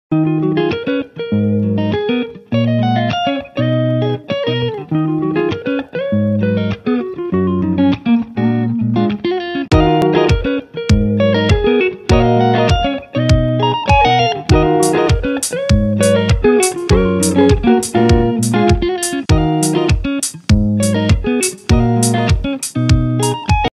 Nod simplu la cravată, în mai puțin de 20 de secunde
Cum să faci un nod simplu, în mai putin de 20 de secunde. 👌

📍 Couture For Gentlemen, Oradea, str. Primăriei, nr.23, Bihor